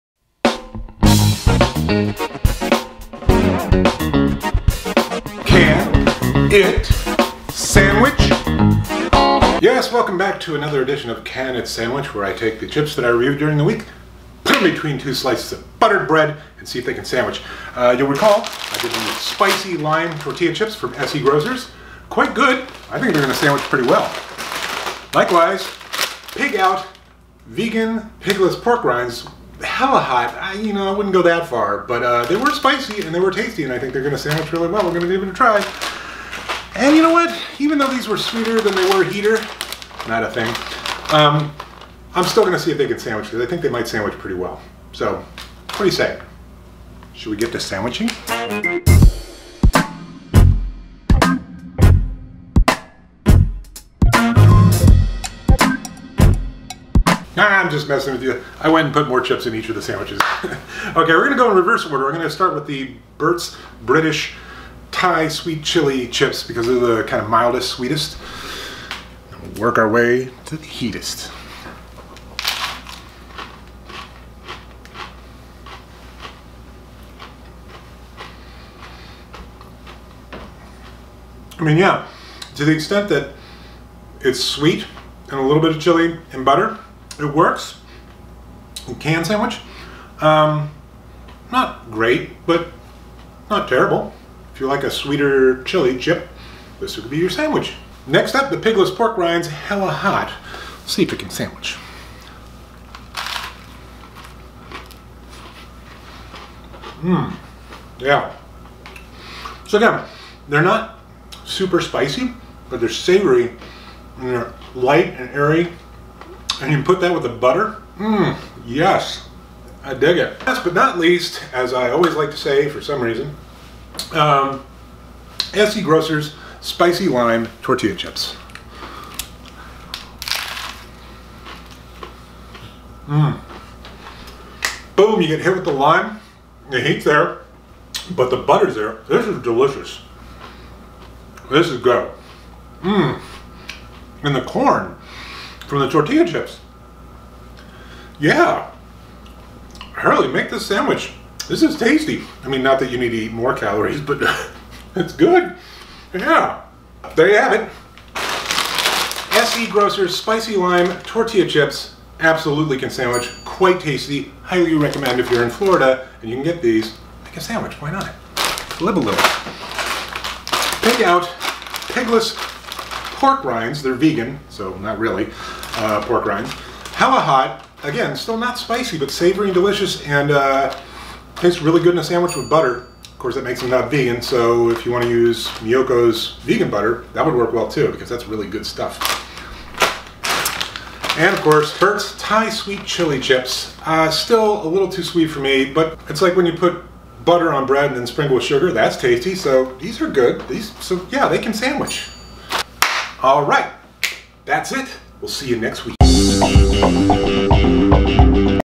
🥪 Can It Sandwich? Spicy Lime/Hella Hot Vegan Rinds/Thai Sweet Chilli - In The Chips with Barry
“Can It Sandwich?”- a Saturday tradition! Yes, this week I see if SE Grocers Spicy Lime Tortilla Chips, Pig Out Pigless Hella Hot Pork Rinds, and Burts Thai Sweet Chilli potato chips can, in fact sandwich.